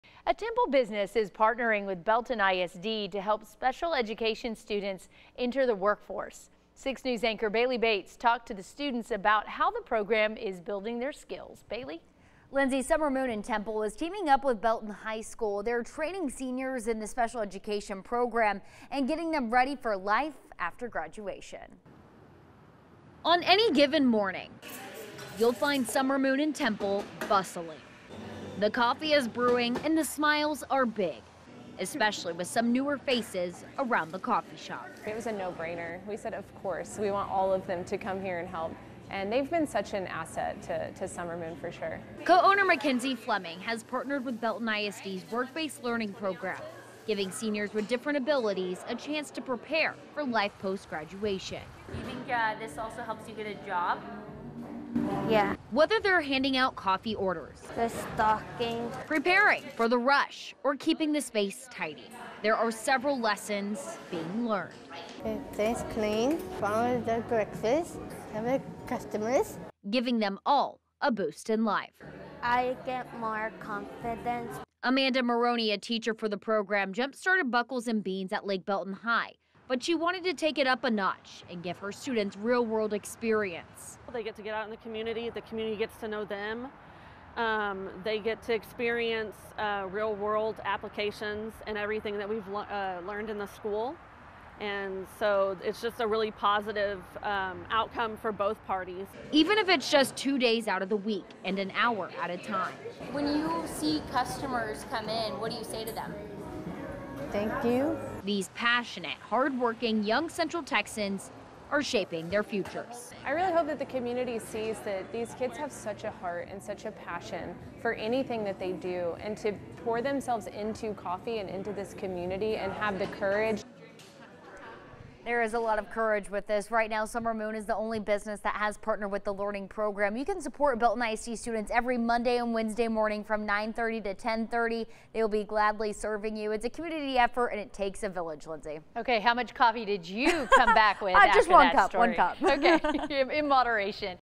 Belton ISD, Summer Moon in Temple partner to help local students
Belton ISD students get hands-on experience at Summer Moon in Temple.